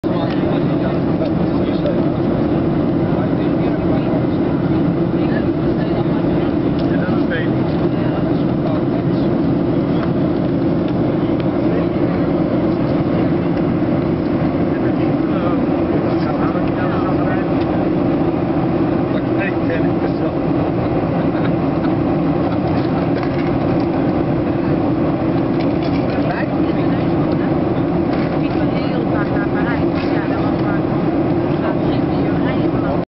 Transavia HV 6115
Aankomst boven Andalucia, Vliegveld Malaga.